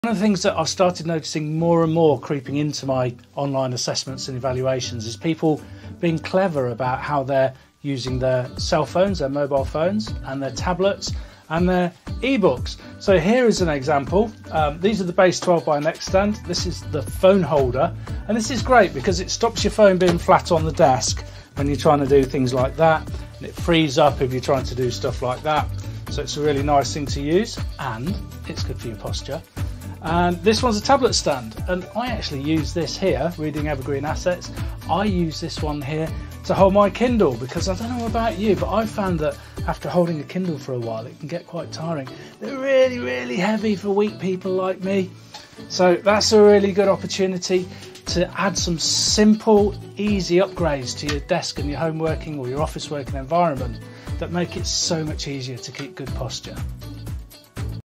Base12 Phone Stand and Tablet Stand Review by James Crow
In this review, James explores the features, design, and functionality of these innovative accessories. Discover how the Base12 stands provide optimal stability, adjustable angles, and sleek aesthetics to enhance your viewing experience. Whether you're a phone addict or a tablet enthusiast, this video is a must-watch for anyone looking to elevate their device setup. Don't miss out on James Crow's expert analysis and insights into the Base12 stands, and make an informed decision for your next tech accessory purchase!"